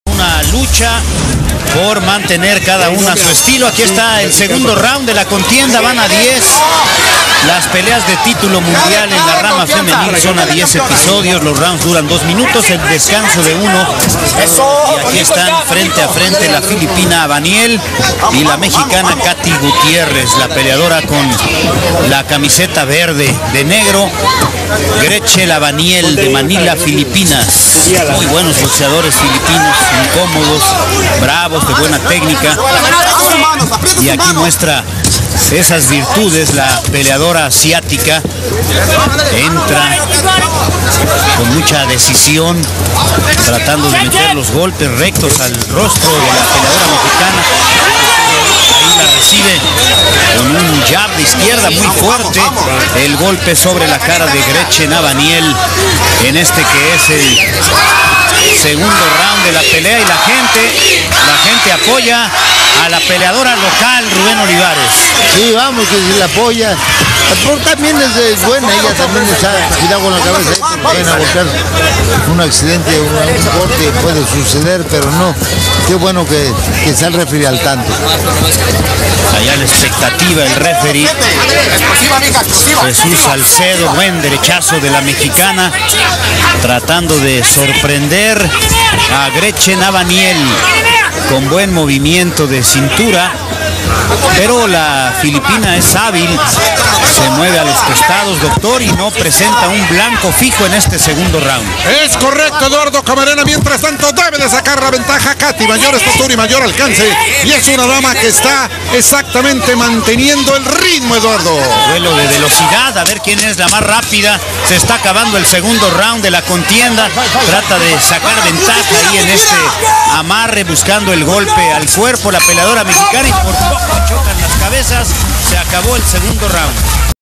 Katia Gutierrez Vs Gretchel Abaniel Round 2
Pelea por el Cinturon Paja de la FIB